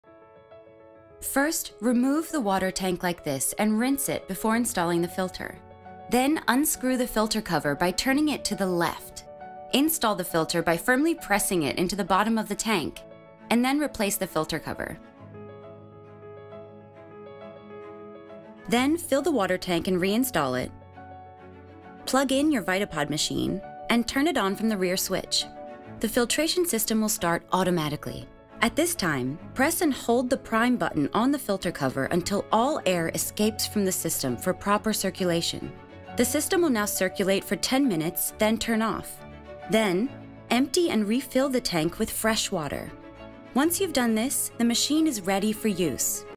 Install the Vitapod Filter
Step 1 of using your Vitapod: Install the filter. This video will take you through exactly how to do it. Remember, the filter needs to be pushed in with a little force before re-installing the filter cover!